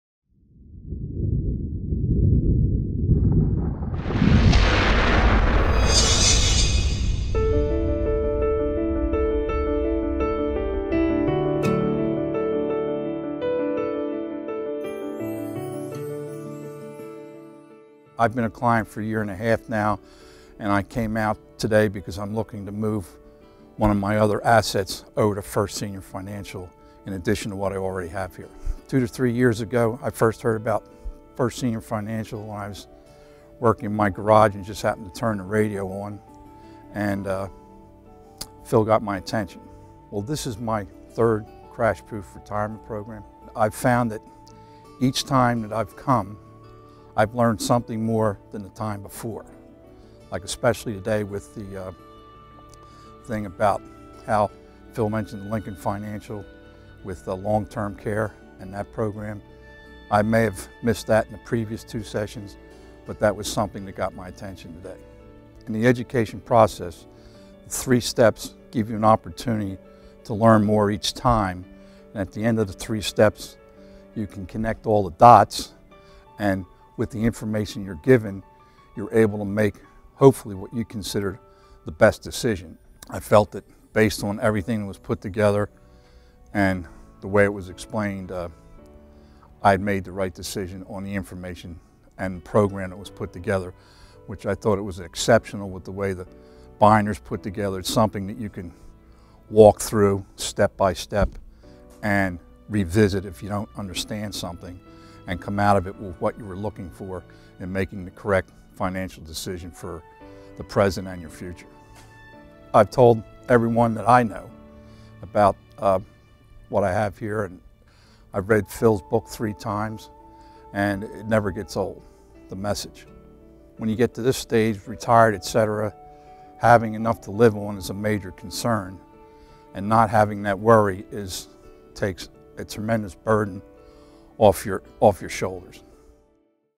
Crash Proof Retirement - Learning New Things All the Time
This consumer has come back over and over to continue his Crash Proof Education, and he learns something new every time... Learn how to get your FREE Crash Proof Education

"I've been a client for a year and a half now, and I came out today because I'm looking to move one of my other assets over to First Senior Financial in addition to what i already have here. Two to three years ago i first heard about First Senior Financial, I was working my garage and just happened to turn the radio on
and Phil got my attention. Well this is my third Crash Proof Retirement program. I found that each time that I've come, I've learned something more than the time before. Like,  especially today with the thing about how Phil mentioned Lincoln Financial with the long-term care and that program I may have missed that in previous two sessions but that was something that got my attention. Today in the education process three steps give you an opportunity to learn more each time. At the end of the three steps you can connect all the dots and with the information you're giving your able to make - hopefully, what you consider the best decision. I felt that based on everything was put together, and the way it was explained, I made the right decision on the information and program that was put together. Which I thought it was exceptional with the way the bynars put together something that you can walk through step by step and revisit if you don't understand something. And come out of it with what you were looking for in making the correct financial decision for the present, and your future. I've told everyone that I know about what i have here and I've read Phil's book three times and it never gets old. The message. When you get to this stage retired etc. having enough to live on is a major concern and not having that worry is takes its immense burden off your off your shoulders."